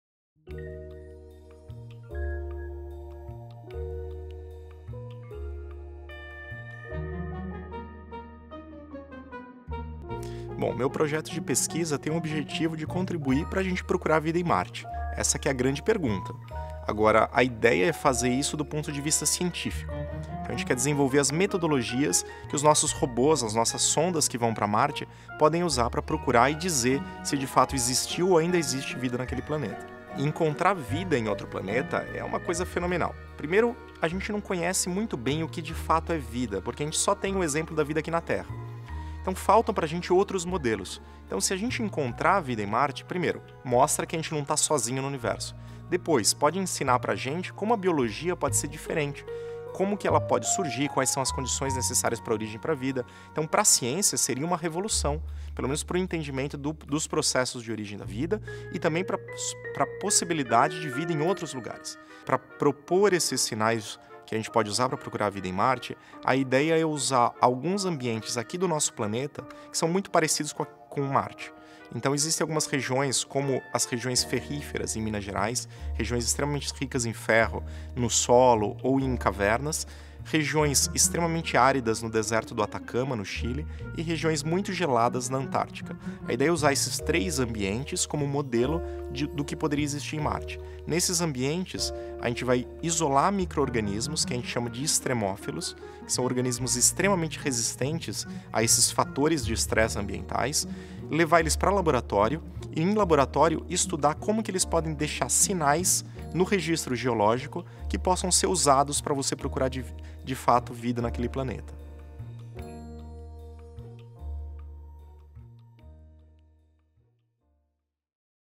Como descobrir se estamos sozinhos no Universo? l Episódio 31 (NÍVEL ll)
Desafiamos o astrobiólogo Douglas Galante a apresentar sua pesquisa para crianças, universitários e especialistas de sua área. Veja aqui a explicação para universitários.

Astrobiólogo do Centro Nacional de Pesquisa em Energia e Materiais, estuda ambientes terrestres extremos análogos a Marte para entender como funcionam os organismos extremófilos que os habitam. Para isso, microrganismos presentes na região ferrífera de Minas Gerais, no deserto do Atacama e na Antártica serão usados para simular sua resposta às condições marcianas. Assim, ele quer contribuir para futuras missões de exploração de Marte e ajudar a responder se estamos sozinhos no Universo.

CRÉDITOS:
Preparação de texto: Humberto e Maria Luisa Lange
Produção: Cadenza Filmes
Direção: Filipe Tomassini
Produção Executiva: Clara Queiroz
Direção de Fotografia: Micael Hocherman
Som direto: Dudu Falcão
Coordenação de pós-produção: Rafael Andreoni
Trilha Sonora: Marcelo Delamare
Edição: João Pedro Sá
Videografismo: Bruno Yoguy
Finalização e cor: Filipe Tomassini e Micael Hocherman